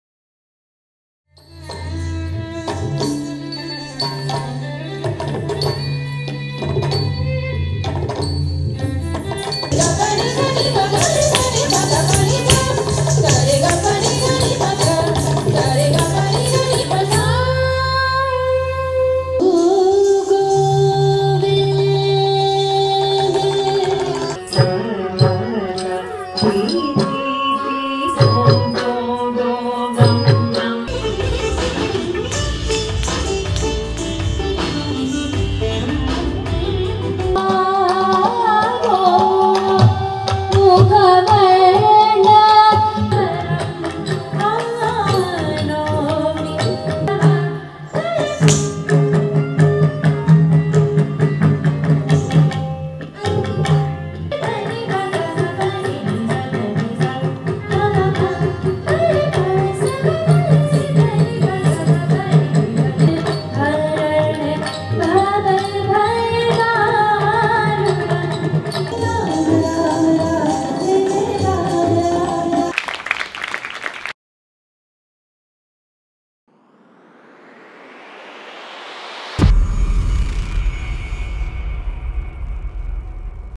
Auroville Festival February 25th: KATHAK & BHARATNATYAM JUGALBAND by Ruchi Sharma & Sujatha Nair
Auroville Festival February 25th: KATHAK & BHARATNATYAM JUGALBAND by Danseuse Ruchi Sharma (Kathak) and Sujatha Nair (Bharatnatyam).

Uttar Dakshin classical music series is one of the known chain of concerts featuring fine nuances of two prominent styles of northern and southern India. Presenting this evening two renowned Danseuse Ruchi Sharma (Kathak)and Sujatha Nair (Bharatnatyam). While Ruchi Sharma is a recipient of Sangeet Natak Akademi awarded Guru and performer, Sujatha is graceful confluence and respected Odissi mohiniattam and Bharatnatyam exponent. We expect a great collaboration of these two artists on the day.